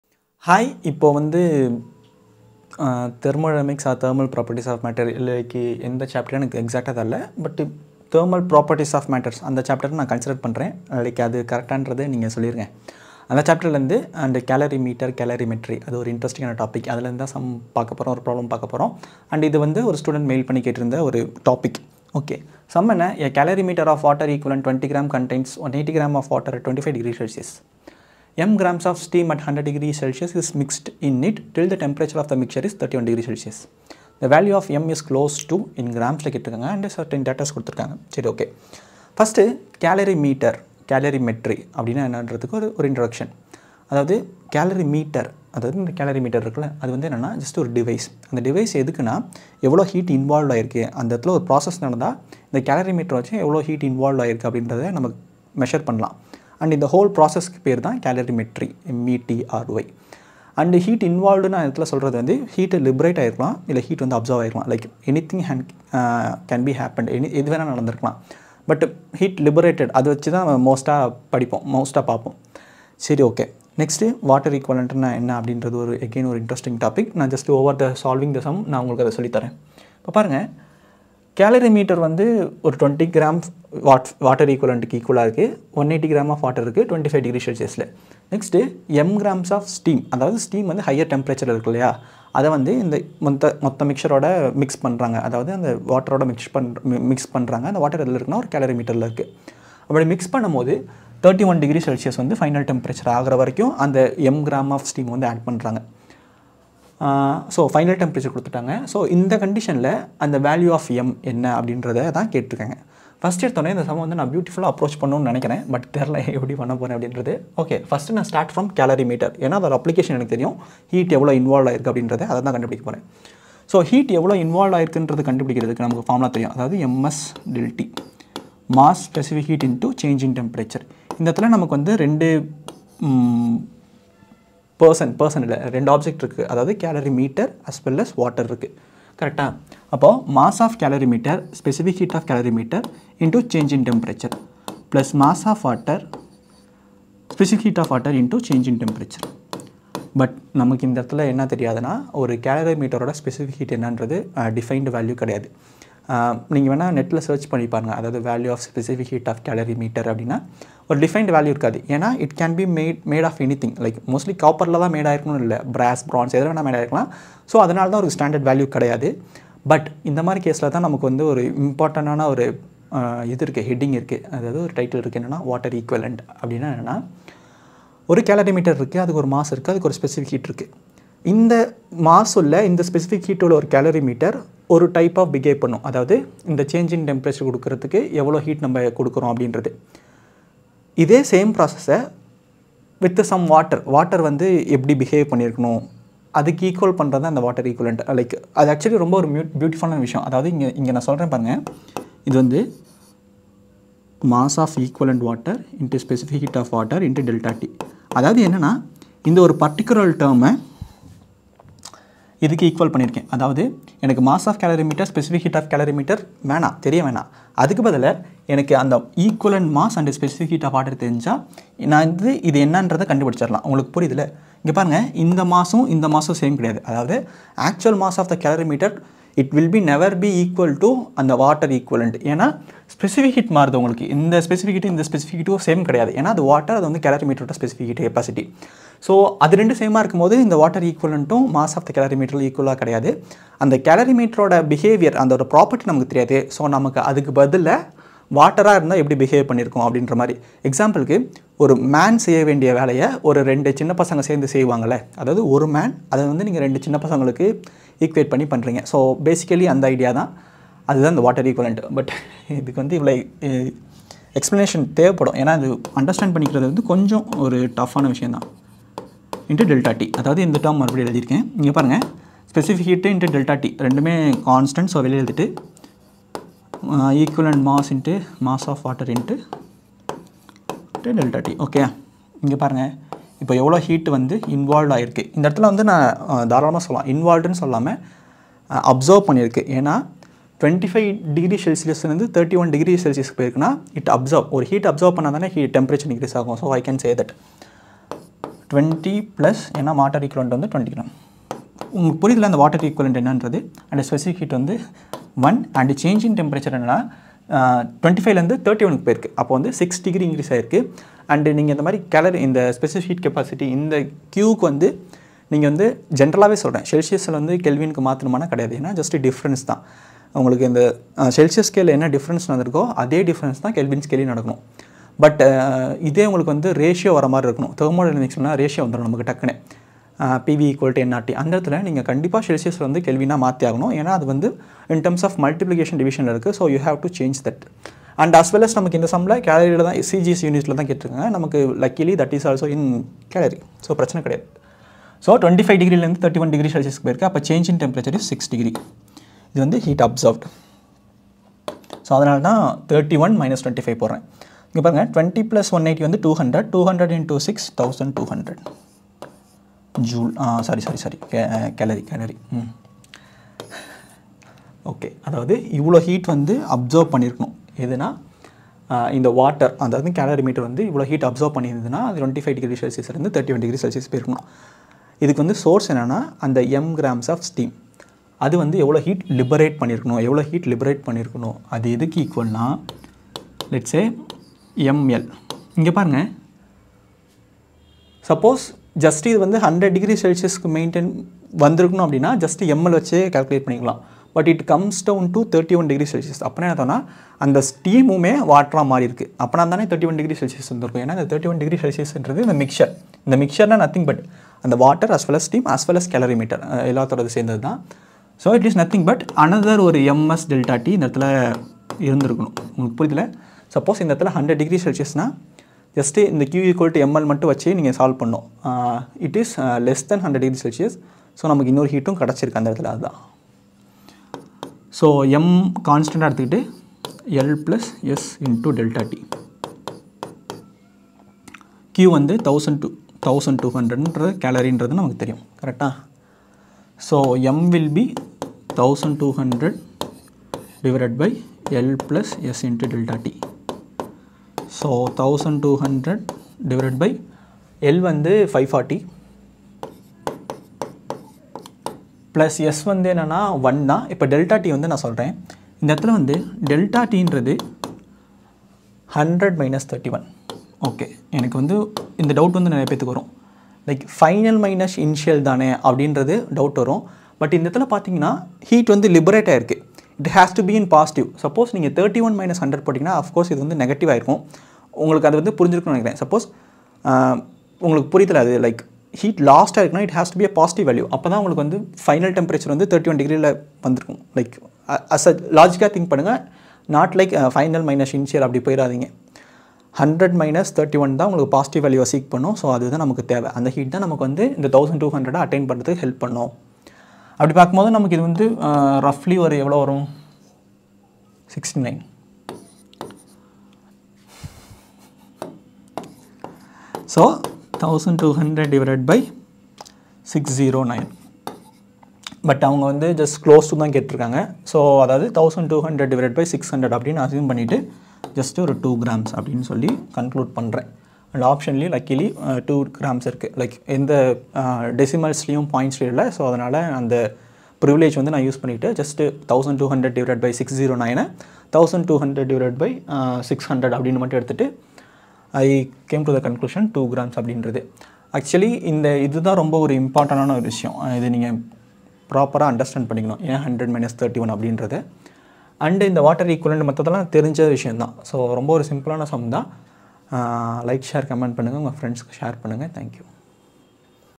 Calorimeter|Thermal properties| NEET JEE Physics Tamil|Problem solving|Latent heat|Water equivalent
Check out this playlist for more NEET JEE based physics problems